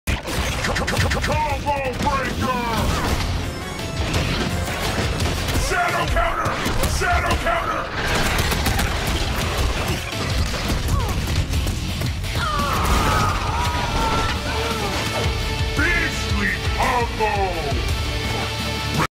Impossible Shadow Counter
I can understand not being able to counter light shorts but, this is a triple overhead command normal.